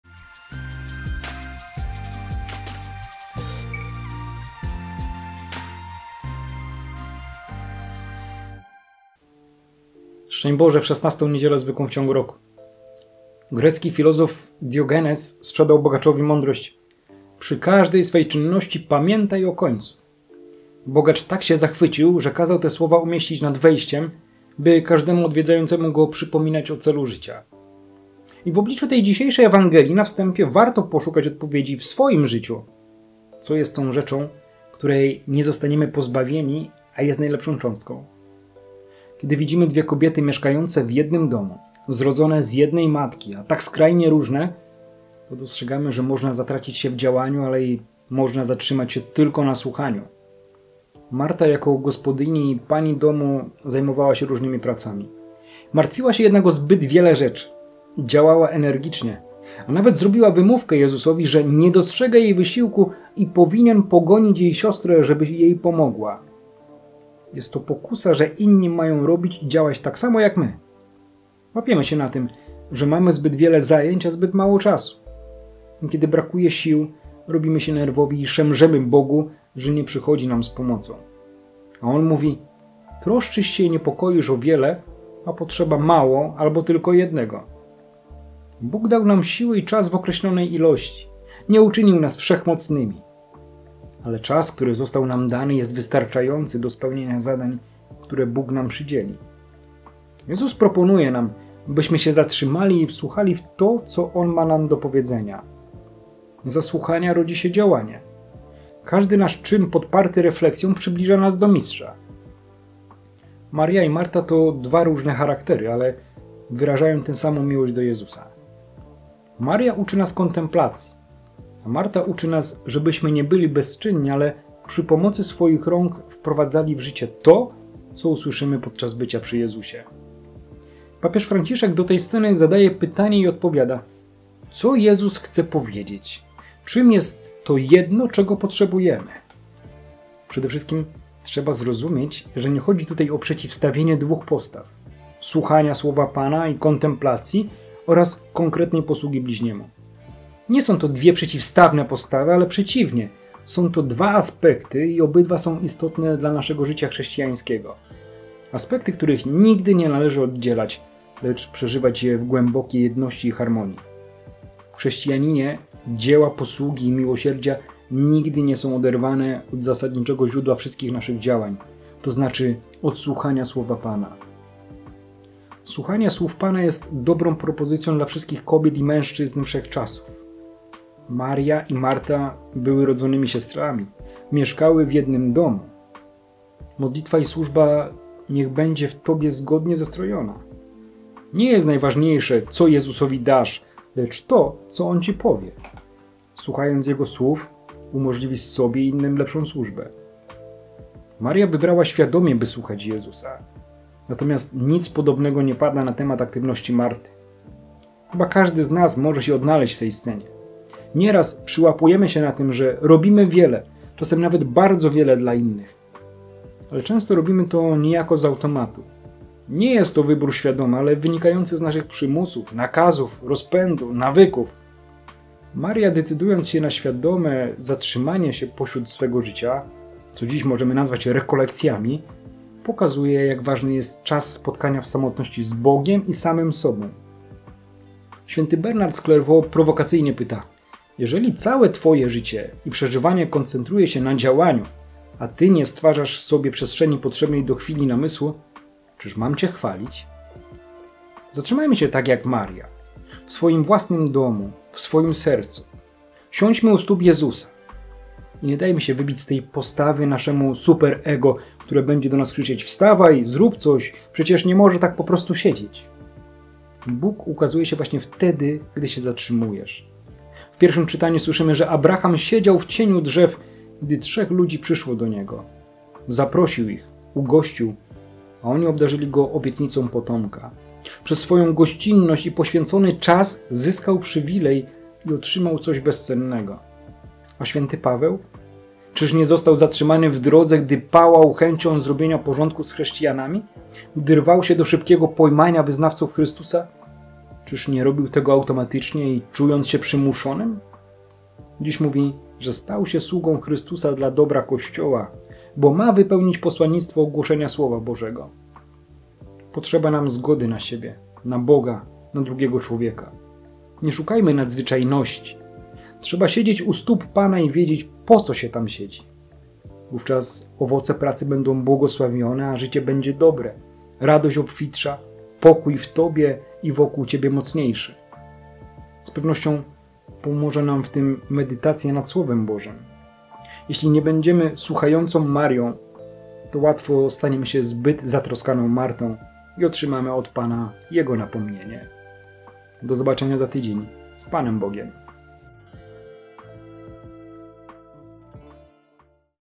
XVI Niedziela Zwykła rok C (Łk 10, 38-42)
XVI Niedziela Zwykła (Łk 10, 38-42)
Nie szukajmy nadzwyczajności. Trzeba siedzieć u stóp Pana i wiedzieć, po co się tam siedzi. Wówczas owoce pracy będą błogosławione, a życie będzie dobre, radość obfitsza, pokój w tobie i wokół ciebie mocniejszy.